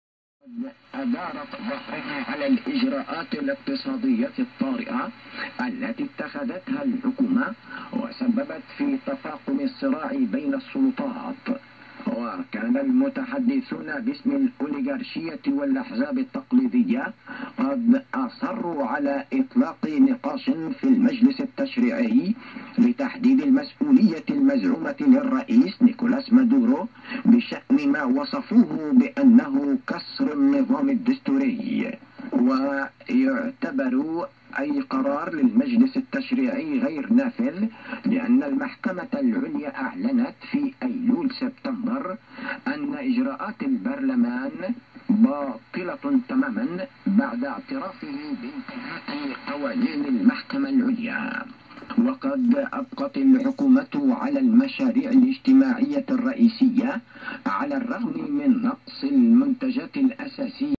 Sultanate of Oman on 15140kHz at 1915 UTC
Received with RTL-STR, 9:1 homebrew UNUN, and long wire